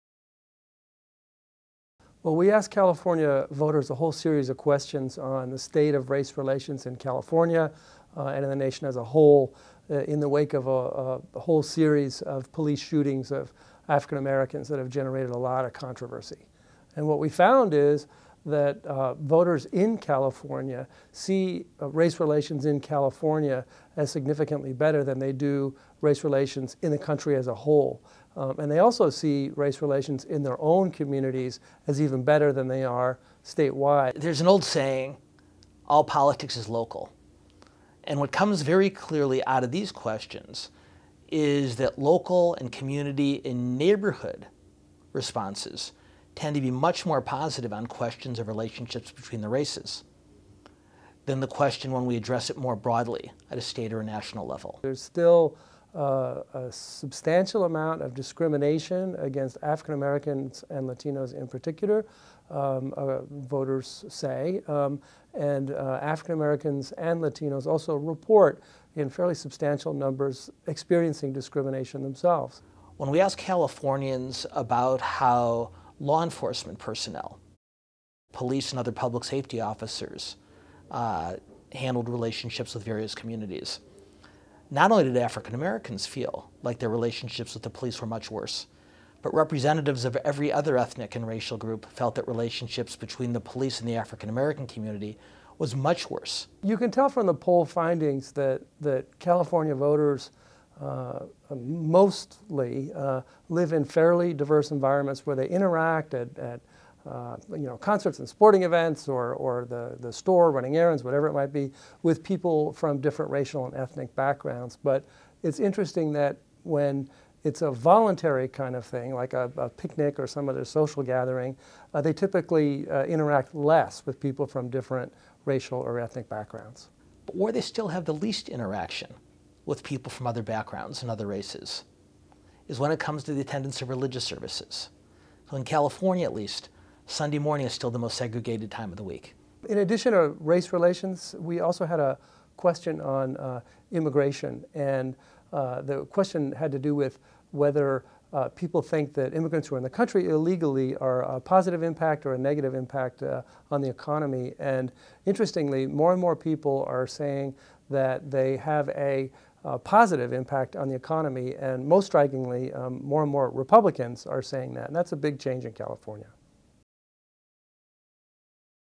Race Relations in California: Poll Results April 2015
The USC Dornsife/ Los Angeles Times Poll is one of the largest public opinion survey polls of California voters. Dan Schnur, director of the Jesse M. Unruh Institute of Politics at USC Dornsife, and Los Angeles Times staff writer, Michael Finnegan discuss respondents' views on race relations in California. For more information on the polls and poll results, please